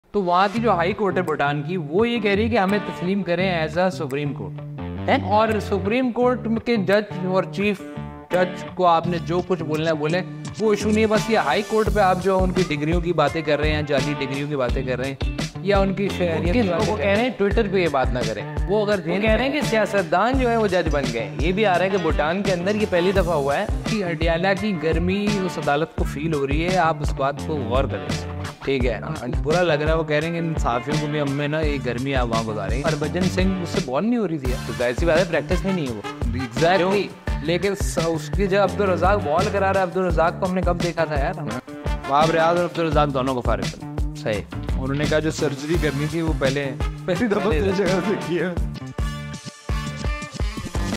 Agar X Band Hai To Judges Per Tankeed Kase Ho Rahi Hai? | Mustafa Chaudhry | Khalid Butt | Promo
Mustafa Chaudhry and Khalid Butt are discussing that If X (Twitter )is a band,  Then how are the judges being criticised?